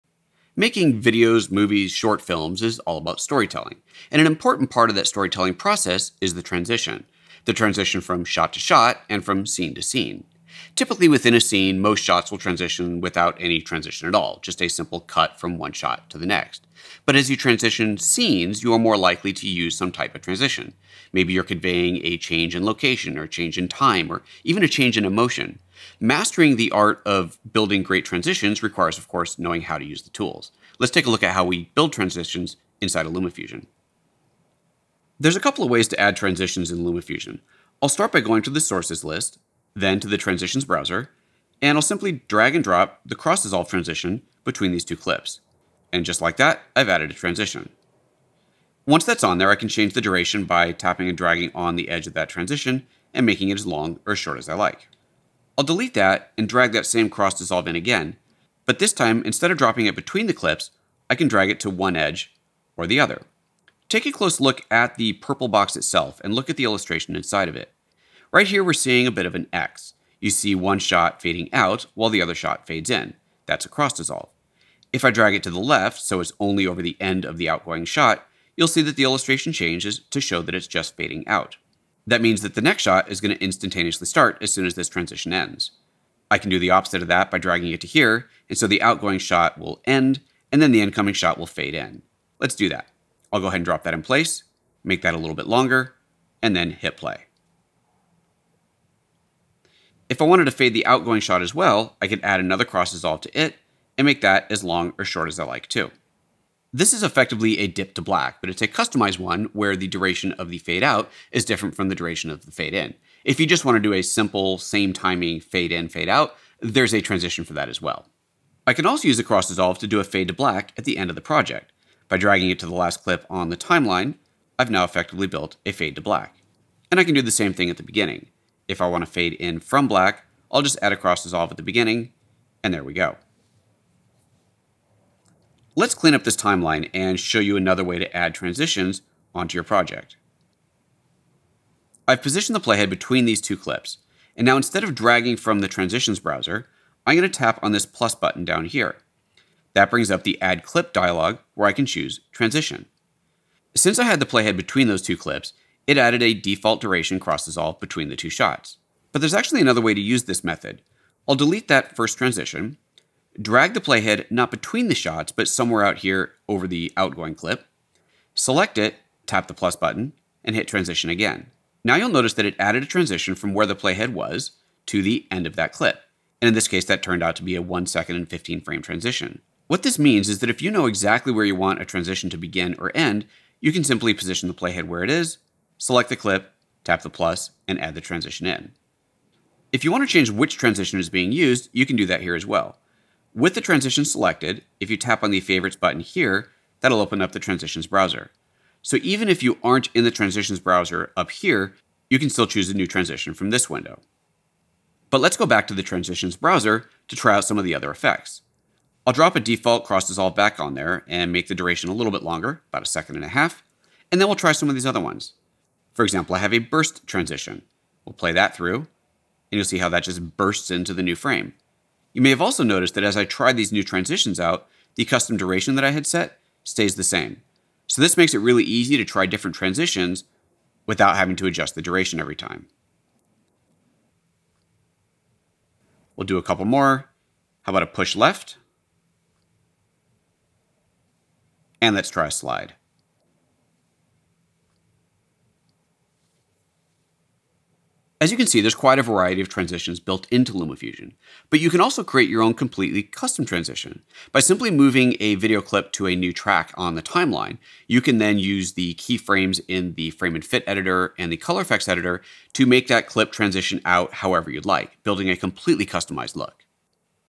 13-1 Transitions; Adding, Positioning, Replacing, and Setting Timing in LumaFusion
In this chapter, you'll learn about Transitions.

Read the LumaFusion reference guide for more information on each tutorial

Presented by Luma Touch and PhotoJoseph.